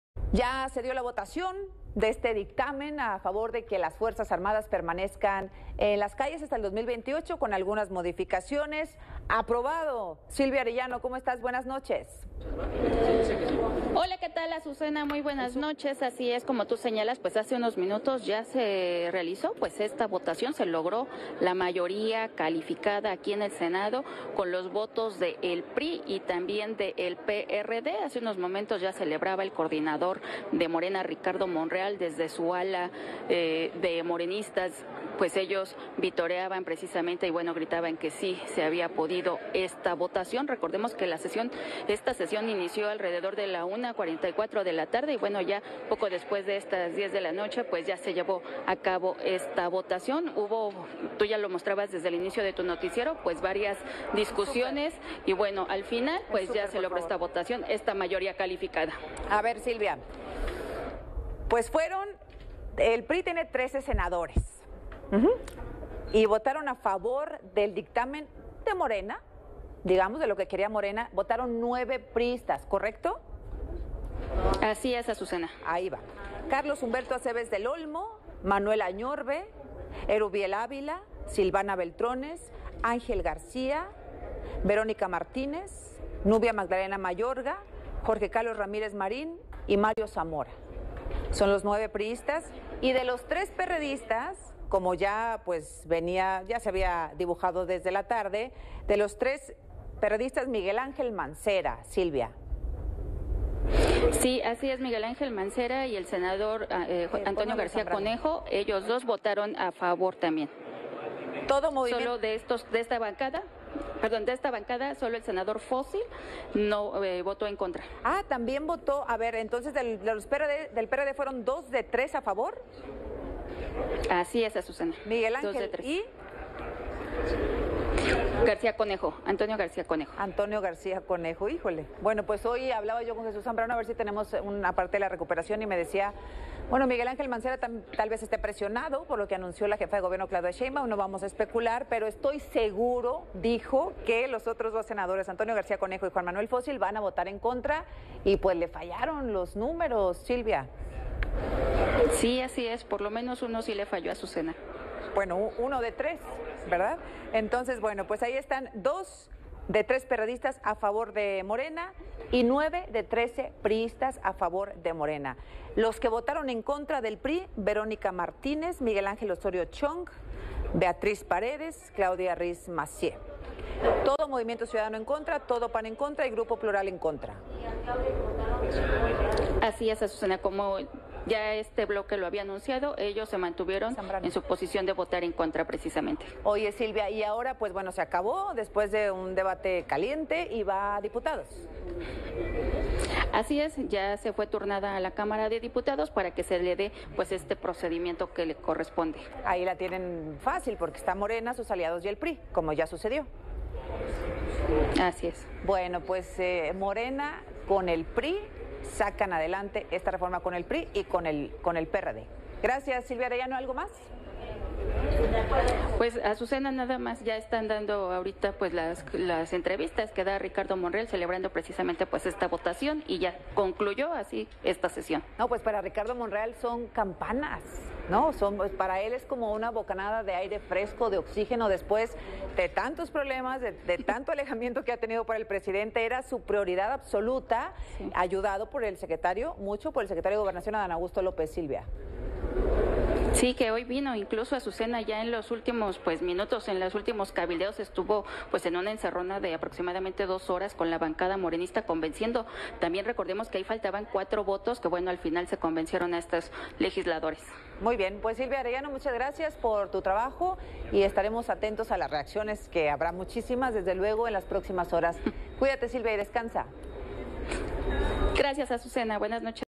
Senado aprueba ampliar las labores del ejército en materia de seguridad pública hasta 2028
AzucenaALas10 | Con 87 votos a favor, 40 en contra, el senado aprobó el dictamen modificado que amplía las labores del ejército en materia de seguridad pública hasta 2028.

Silvia Arellano nos tiene el reporte

¿Qué senadores de la oposición votaron a favor?